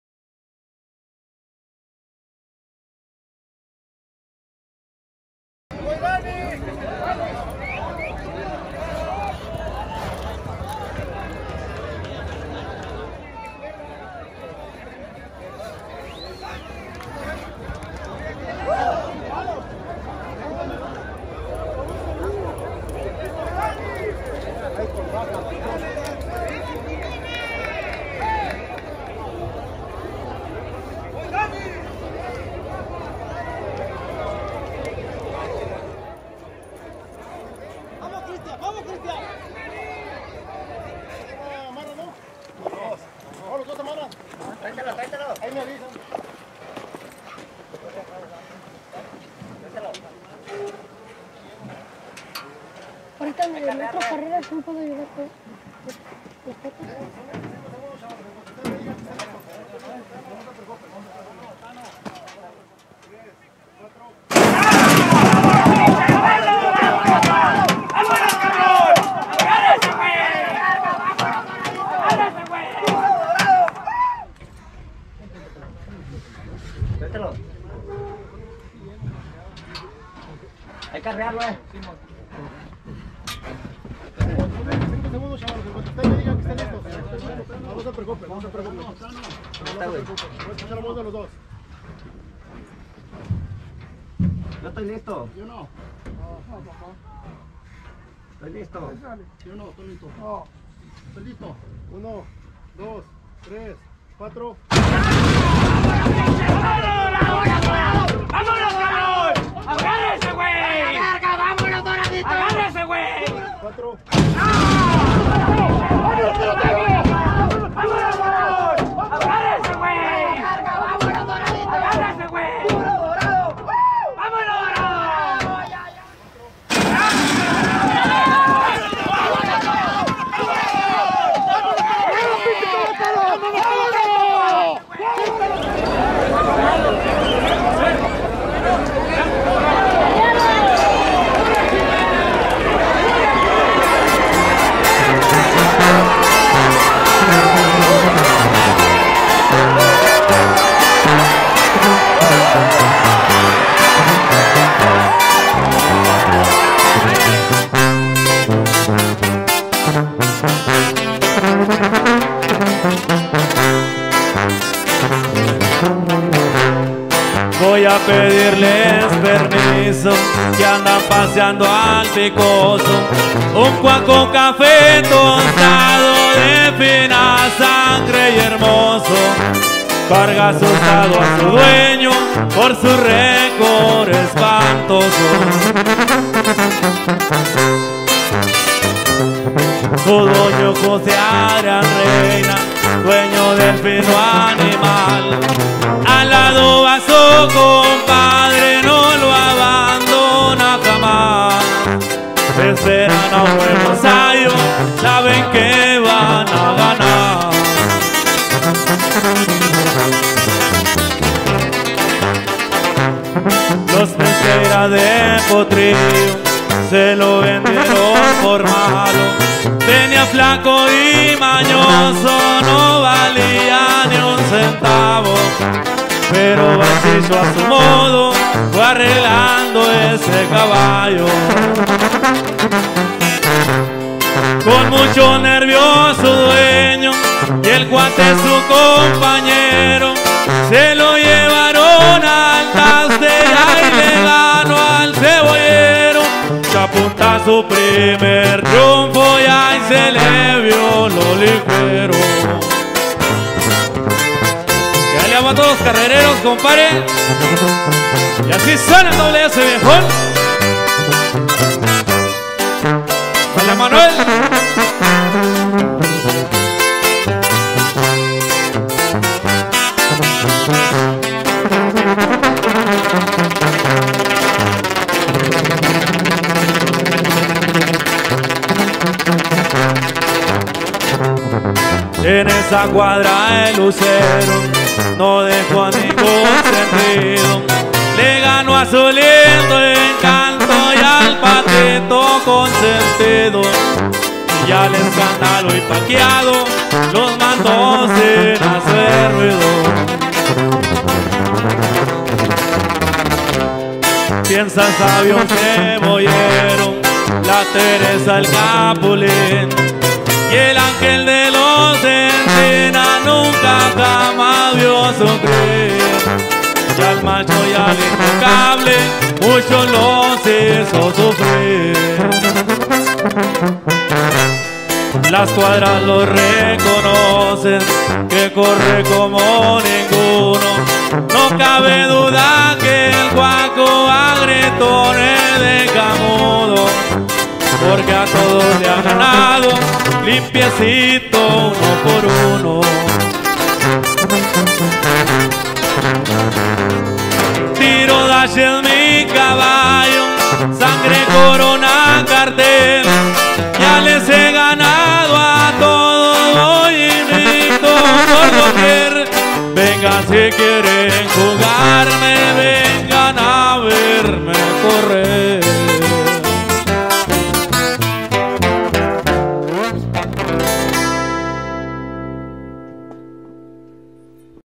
El Pelotero Cuadra DannyBoy vs El Dorado Cuadra Jimena
El Pelotero Cuadra DannyBoy vs El Dorado Cuadra Jimena en Caril Moapa Equestrian Sport de Las Vegas, Nevada.